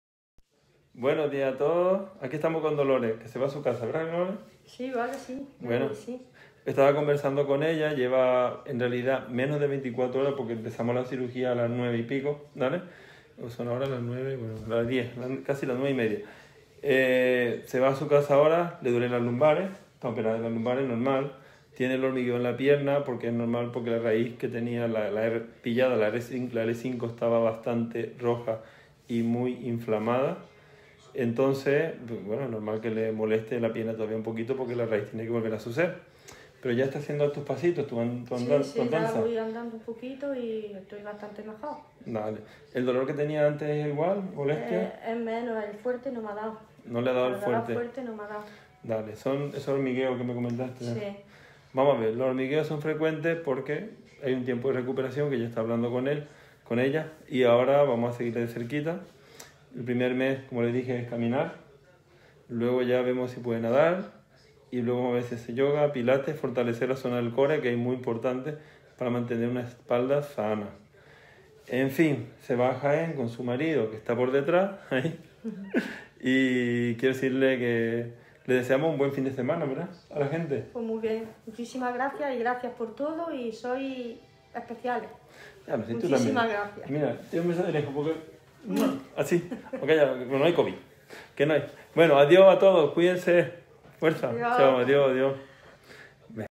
Dolores está de alta - Clínica Dr. Mosqueira
Buenos días amigos, A 24 horas de su intervención por una estenosis de canal lumbar L4-L5, Dolores está de alta.